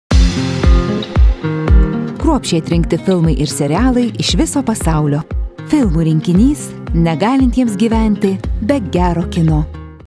Papildomas TV kanalų rinkinys FILMŲ
Tai TV kanalų rinkinys, skirtas tiems, kad negali gyventi be gero kino. Rinkinys retransliuojamas įvairiomis kalbomis. Jį sudaro 6 kanalai.